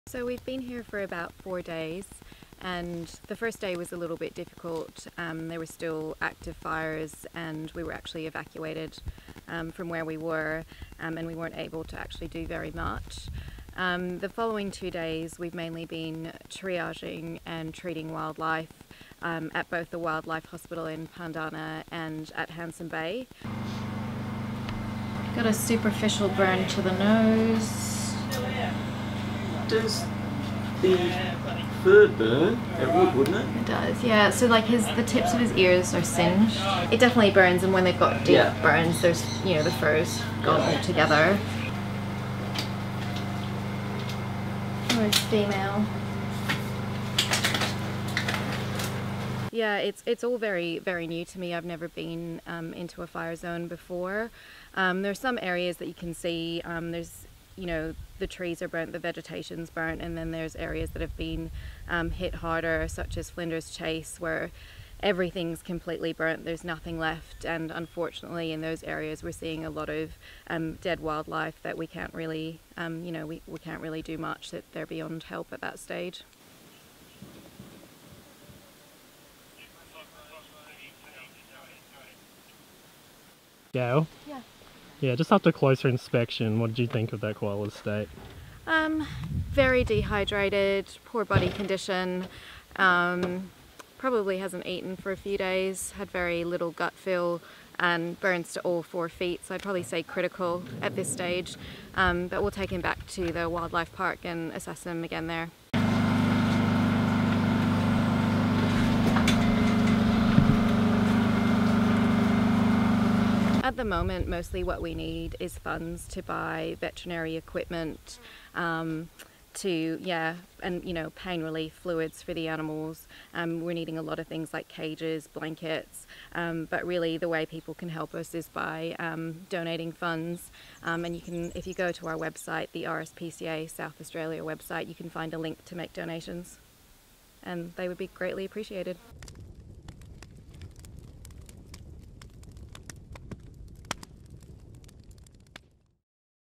Kangaroo Island bushfires - first-hand account from RSPCA vet Gayle
RSPCA veterinarian Gayle has just returned from four days on Kangaroo Island, helping triage and treat dozens of animals who survived the intense bushfires.